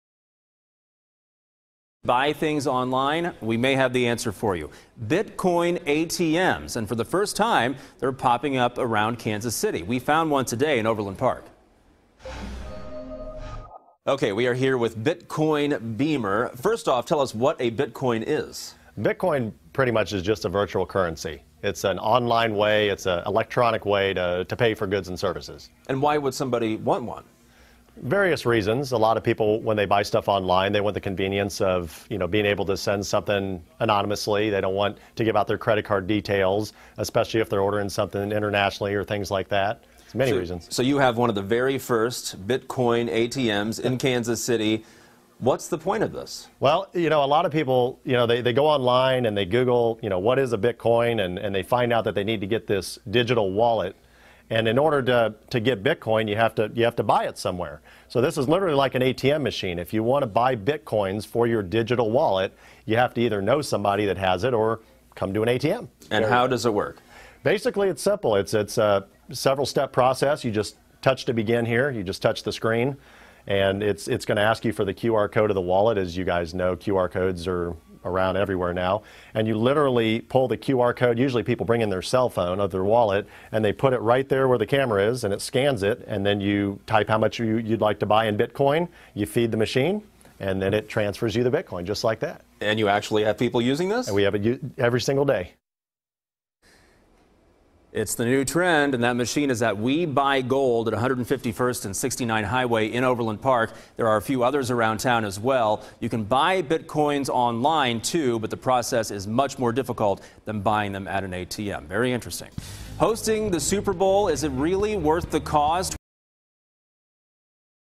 First Bitcoin ATMs popping up in Kansas City
41 Action News, KSHB, brings you the latest news, weather and investigative reports from both sides of the state line.